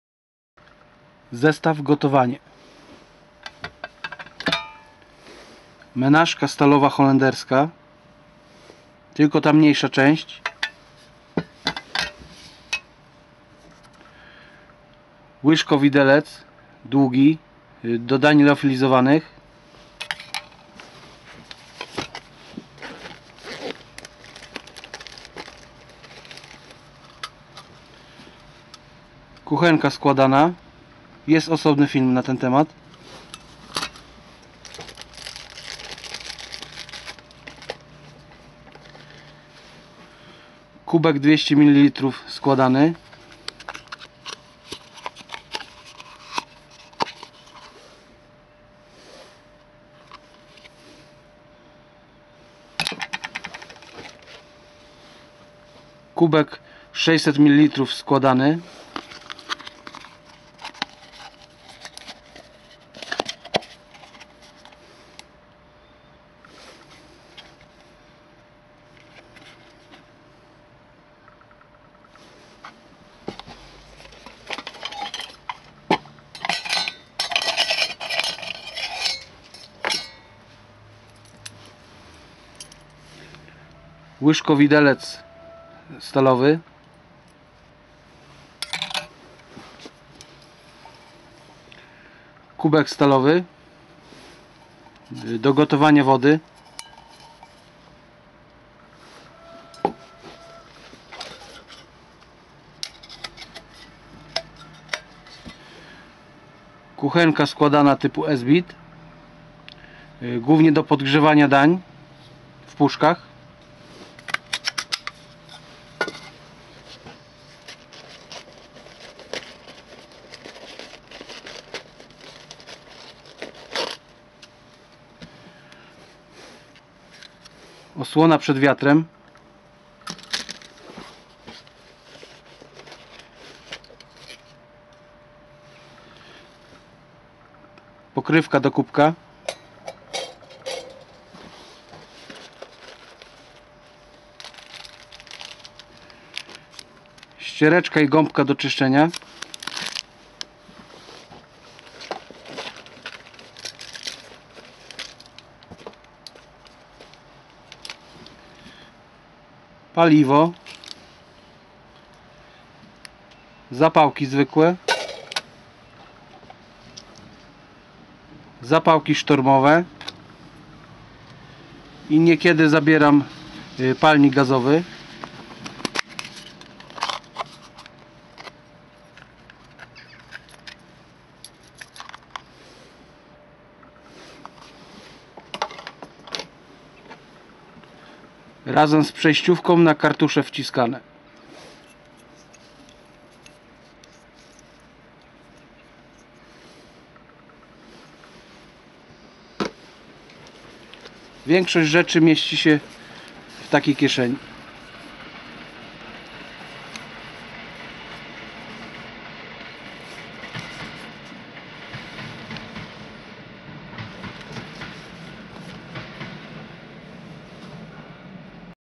Plecak wyprawowy - gotowanie
Plecak wyprawowy.

Zestaw gotowanie:
- menażka stalowa holenderska mniejsza część
- łyżko-widelec plastikowy długi do dań liofilizowanych
- kuchenka składana na różne paliwa
- kubek składany 200ml
- kubek składany 600ml
- łyżko-widelec stalowy składany
- kubek stalowy 500ml ze składanymi rączkami
- kuchenka składana typu Esbit na paliwo stałe
- osłona przed wiatrem
- pokrywka do kubka stalowego
- ściereczka i gąbka do czyszczenia
- paliwo stałe typu Esbit
- zapałki zwykłe
- zapałki sztormowe
- palnik gazowy z przejsciówką na kartusze wciskane
- kartusz gazowy (brak na filmie)